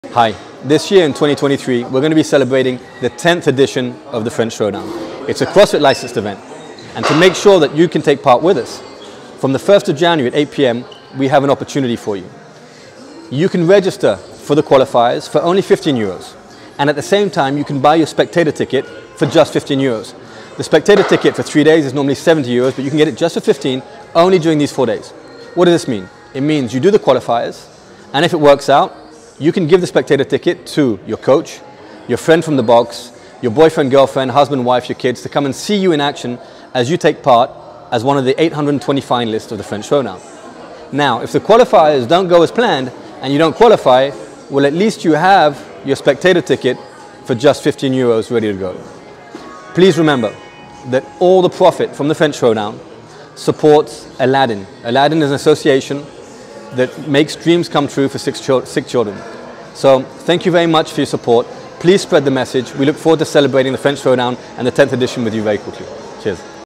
French Throwdown 10th anniversary offer.
For all qualifiers registrations next January 01, 2023, you will have the opportunity to take advantage of a 3-day nominative "Spectator Pass" for only 15€ (instead of 70€ public price)

👉🏼 If you are lucky enough to qualify for the grand finale, this pass can be used for your companion.
👉🏼 If unfortunately the qualification is not for this year, come and console yourself and use your spectator pass to come and watch the competition.
‼️ WARNING : this "special anniversary offer" cannot be purchased separately from your qualification registration. So, check twice before validating your cart 😉